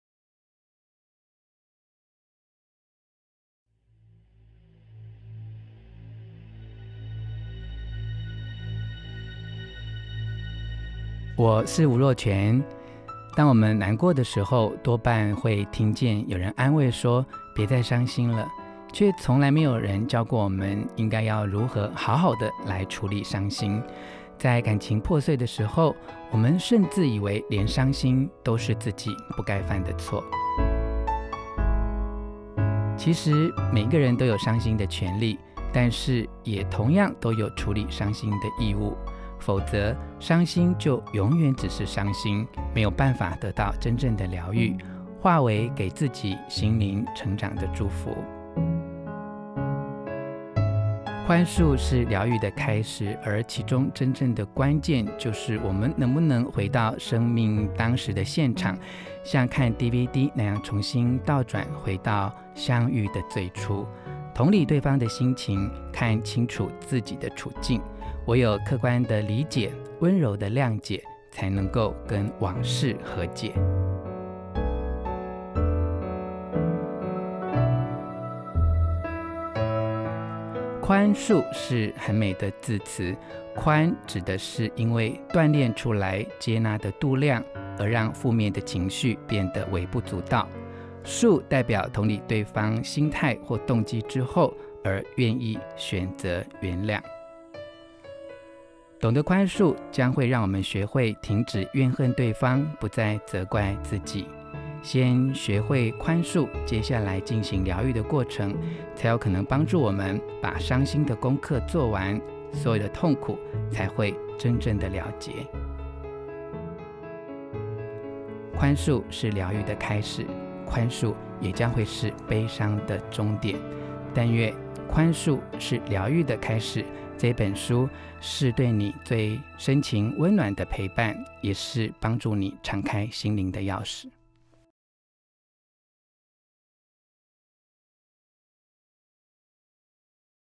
吳若權《寬恕是療癒的開始》
往事，像一座旋轉木馬，
當我看著前面的風景說「那些都過去了」；
內心卻還是無法停歇的繼續複習，
我慢慢知道，療癒自己最好的方式並非忘記，
也不是重複之後的麻痺，而是對已發生過的一切，心存感激。

吳若權最新作品《寬恕是療癒的開始》用深情和溫暖鑄成一把鑰匙，
打開心扉那道緊閉的鎖，
陪伴我們在受傷後，勇敢地往下一段幸福前進！

2012年11月28日　溫暖上市